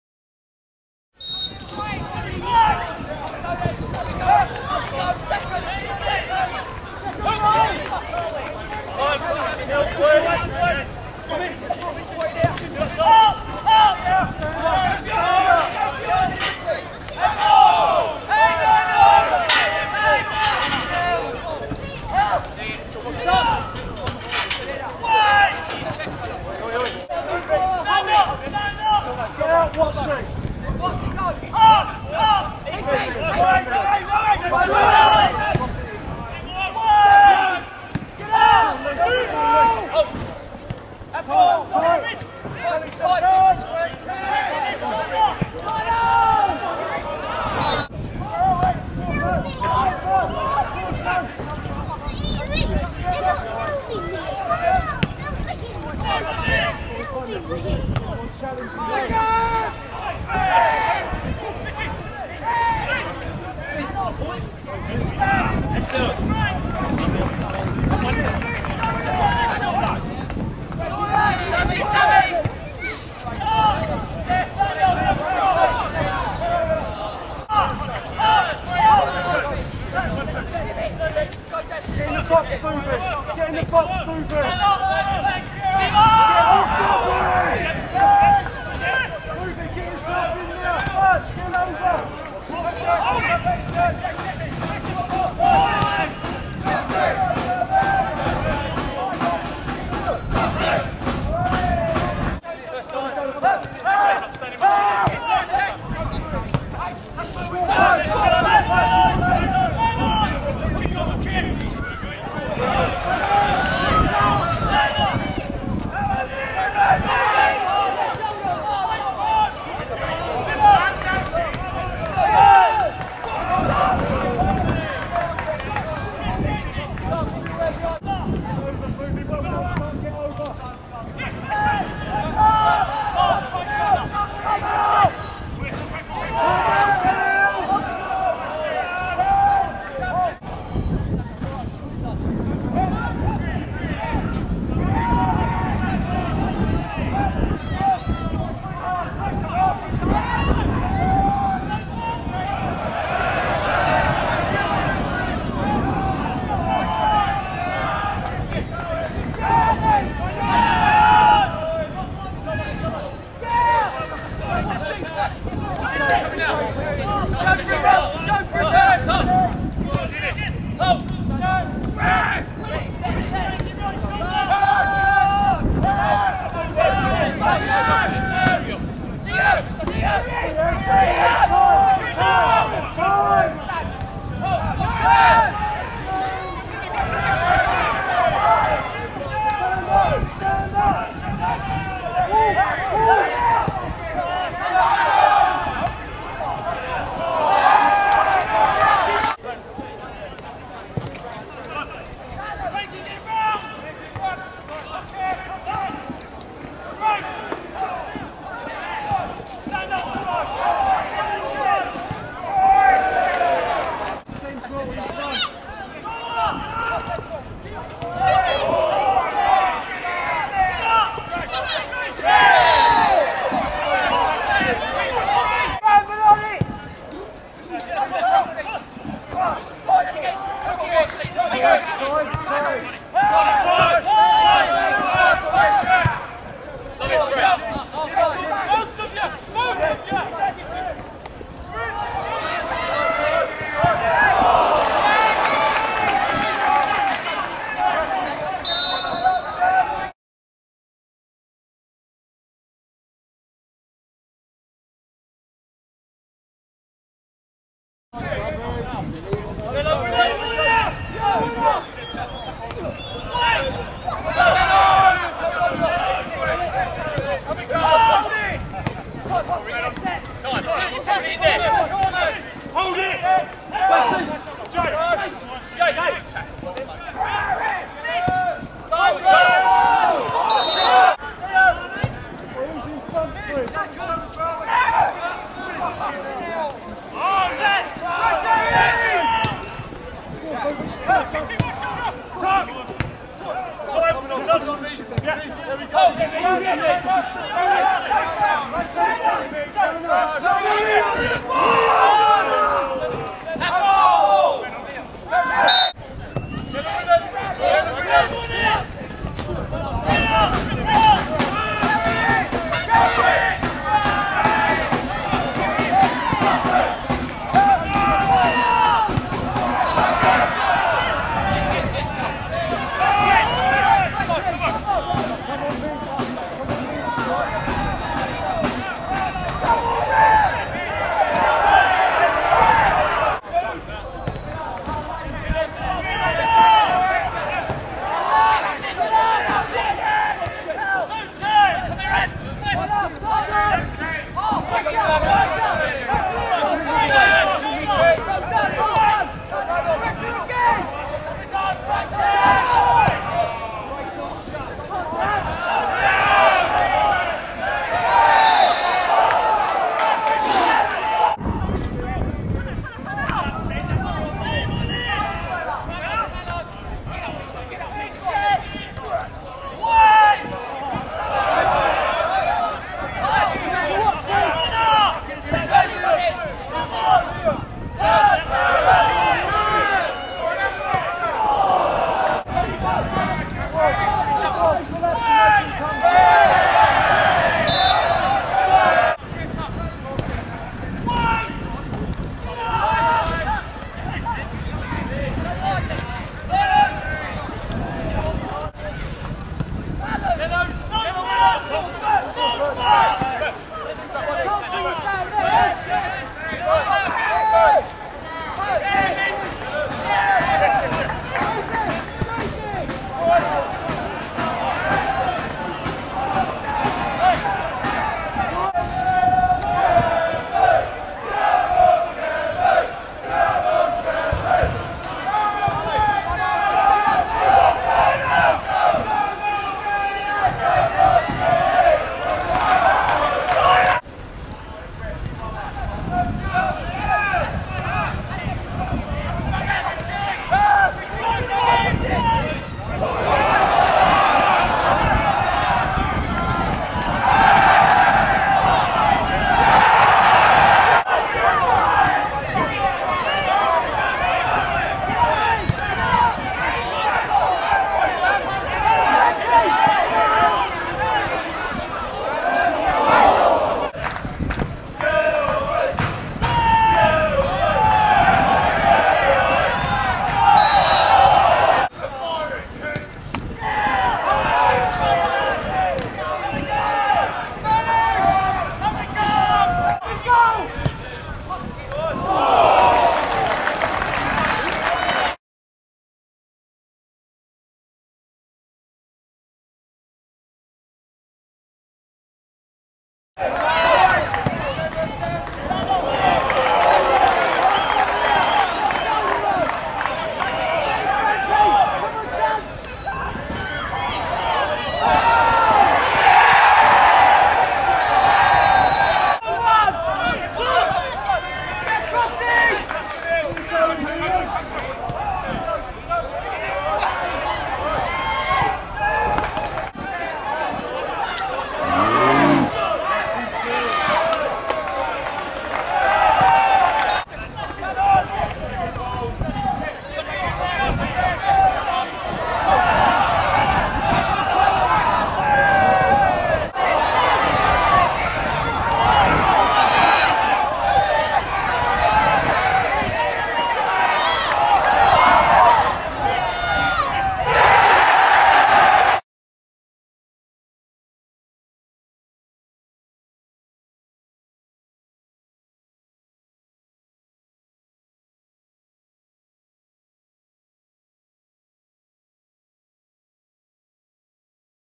TONBRIDGE ANGELS V CANVEY 2ND HALF HIGHLIGHTS
2nd Half Highlights. Tonbridge Angels ran out fine winners 3-1 with a last gasp 3rd goal. A hard fought victory.